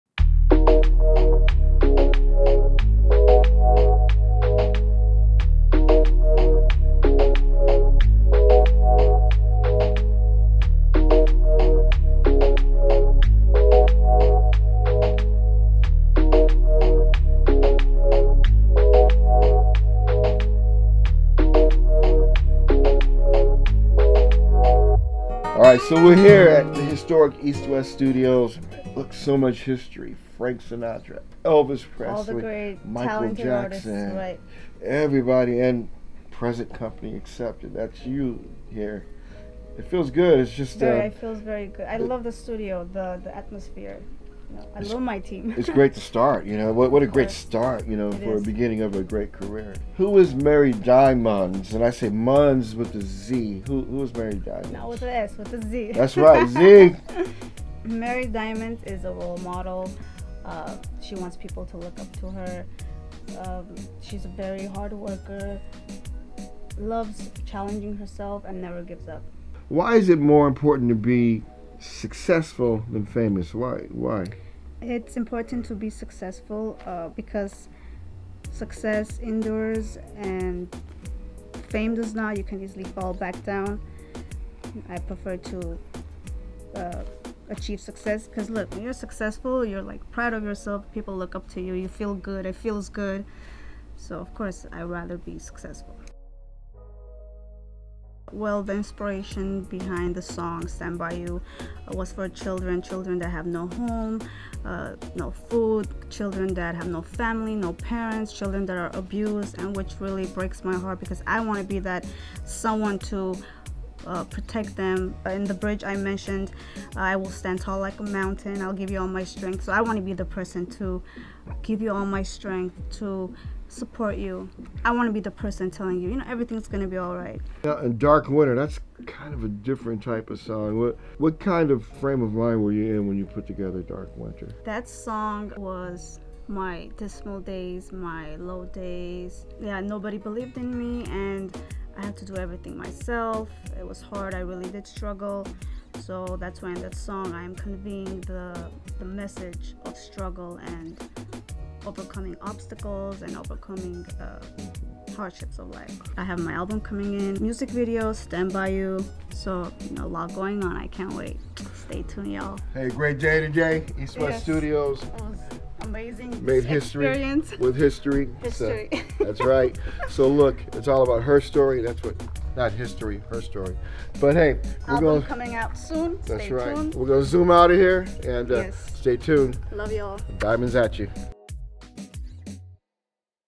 Behind the scenes documentary with Mery Diamondz and Andrew Lane!! EastWest Studios Hollywood, CA.
Behind the scenes documentary with Mery Diamondz and Andrew Lane!!

Mery Diamondz and Andrew Lane take a moment from their studio session to discuss
Mery's artistry, musical goals and everyday passions.

Thank you to EastWest Studios for your amazing hospitality!!

        **CREDITS**
Artist: Mery Diamondz
Producer: Andrew Lane
Musician: Jeremy Lappitt
                  Christian Moraga
                  Nico Aranda

    ***Special Thanks***
Kat Erangey
Miko Harutyunyan
Andrew Lane
Ashley